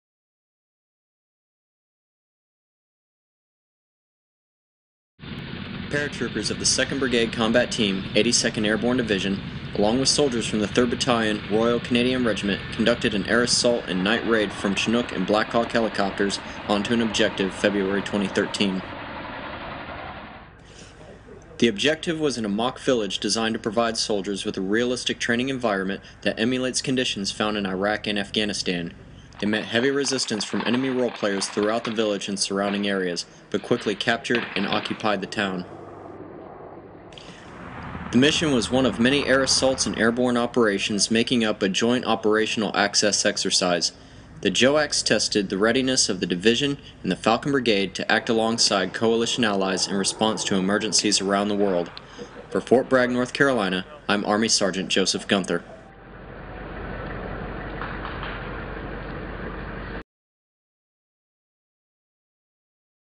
82nd Airborne & Canadian Soldiers Train
Paratroopers with 2nd Brigade Combat Team, 82nd Airborne Division train with 3rd Battalion, Royal Canadian Regiment in Joint Operational Access Exercise Feb. 28, 2013.